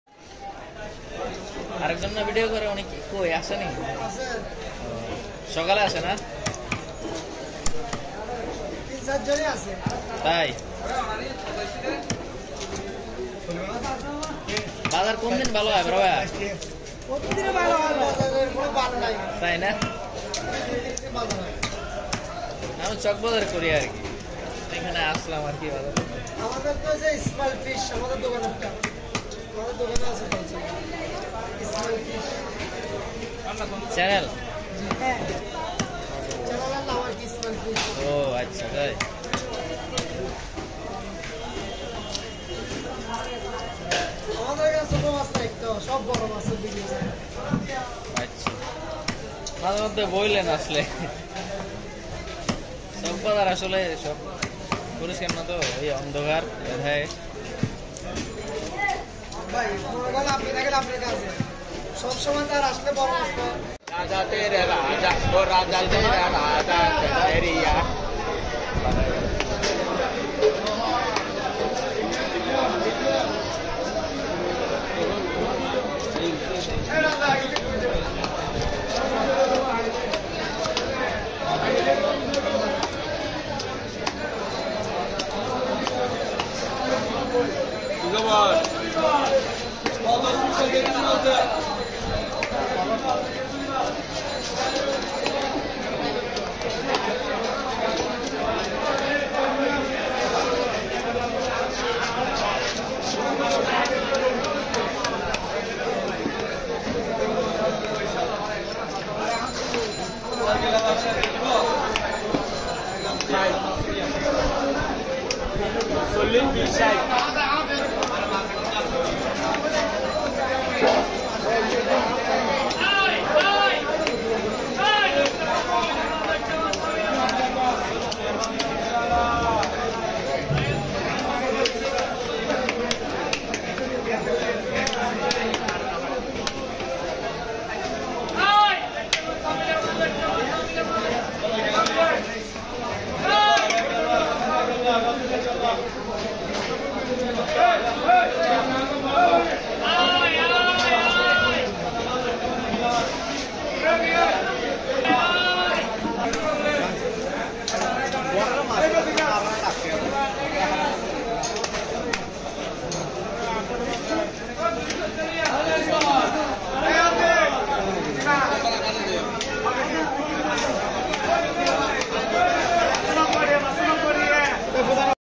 Giant Rohu &Shrimp Fish Cutting Skills Live in Fastest Fish Market
Giant Rohu &Shrimp Fish Cutting Skills Live in Fastest Fish Market 2022